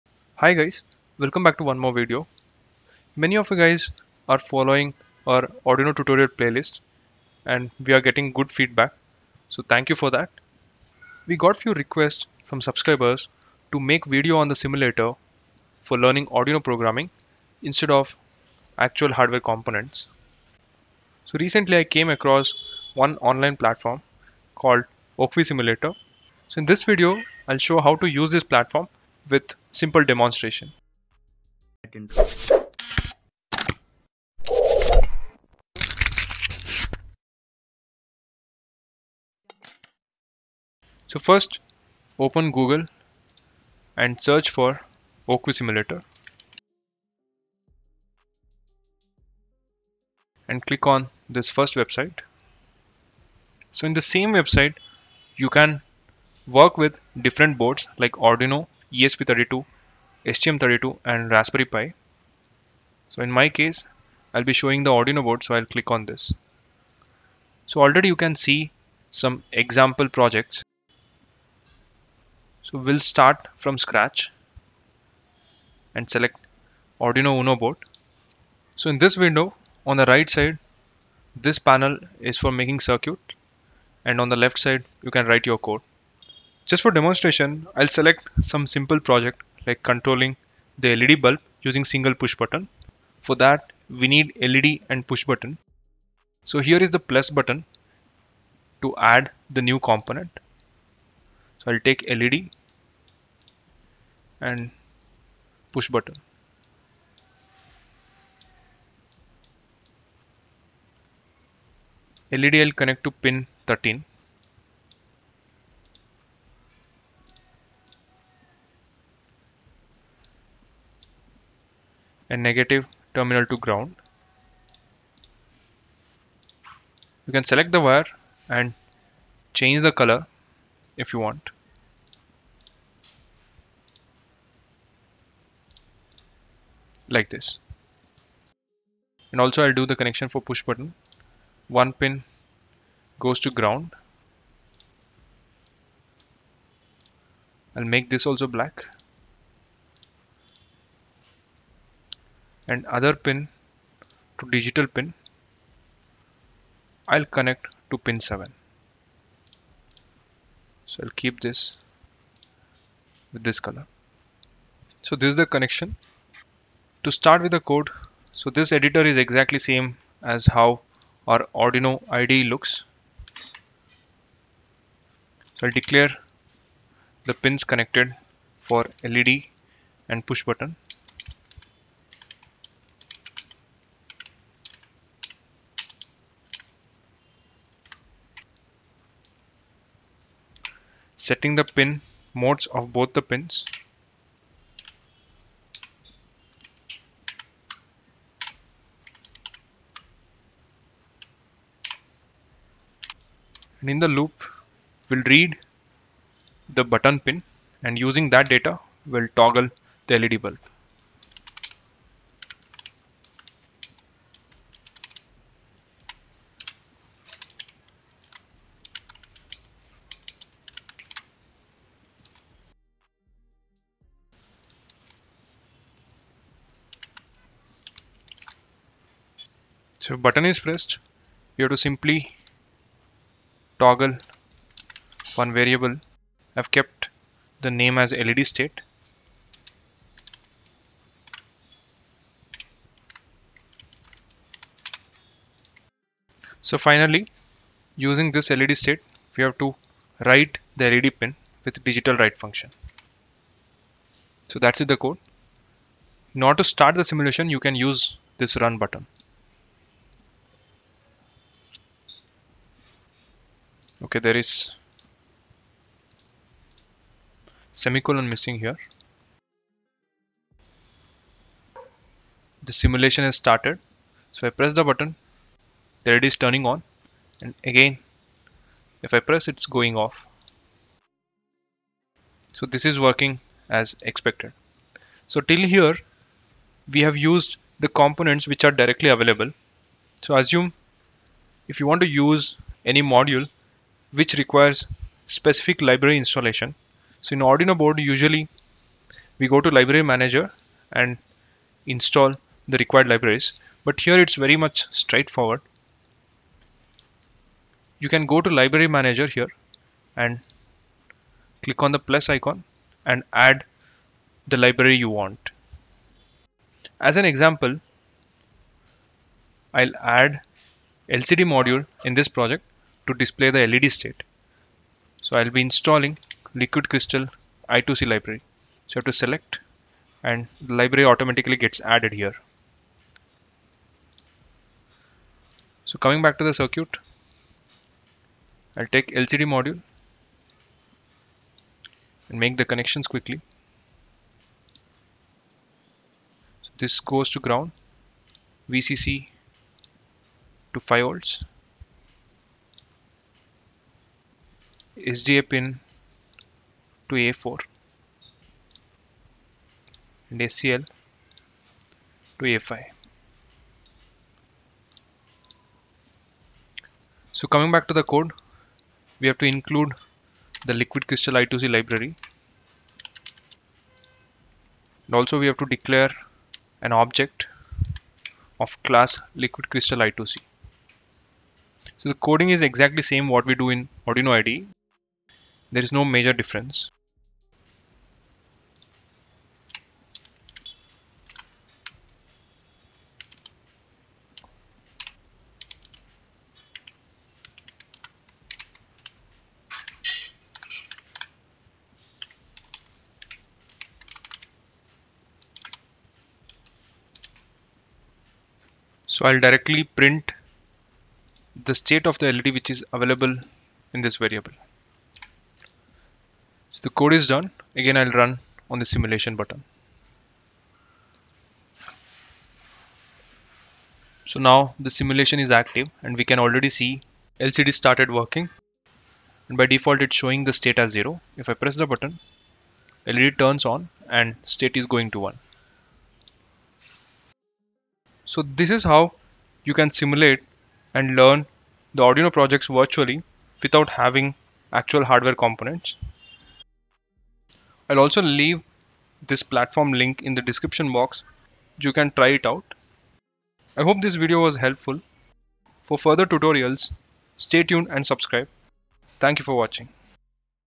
Best Arduino Simulator Online for Arduino Programming | WOKWI Simulator Tutorial for Beginners
Hi Guys,
This video will show you how to learn Arduino programming on Wokwi simulator without using actual components. Hope you like it!!